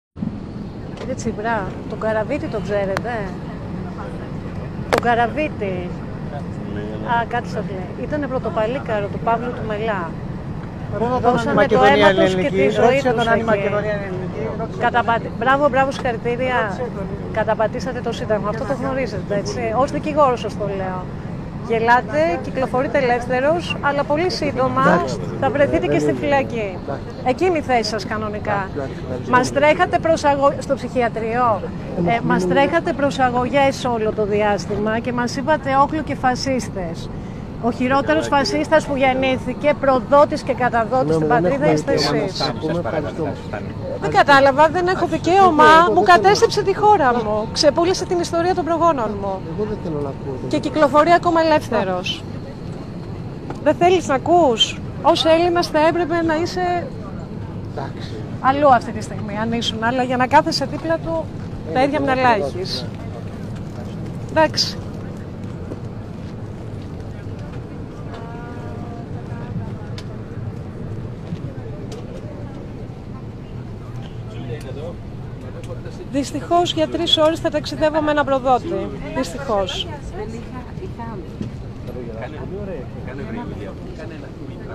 ΦΡΑΣΤΙΚΗ ΕΠΙΘΕΣΗ ΣΤΟΝ ΑΛΕΞΗ ΤΣΙΠΡΑ ΑΠΟ ΤΗ ΔΗΜΟΣΙΟΓΡΑΦΟ ΓΕΩΡΓΙΑ ΜΠΙΤΑΚΟΥ
ΦΡΑΣΤΙΚΗ ΕΠΙΘΕΣΗ ΣΤΟΝ ΑΛΕΞΗ ΤΣΙΠΡΑ ΑΠΟ ΤΗ ΔΗΜΟΣΙΟΓΡΑΦΟ ΓΕΩΡΓΙΑ ΜΠΙΤΑΚΟΥ ΣΕ ΤΕΤ Α ΤΕΤ ΣΤΟ ΑΕΡΟΠΛΑΝΟ ΠΟΥ ΣΥΝΤΑΞΙΔΕΥΑΝ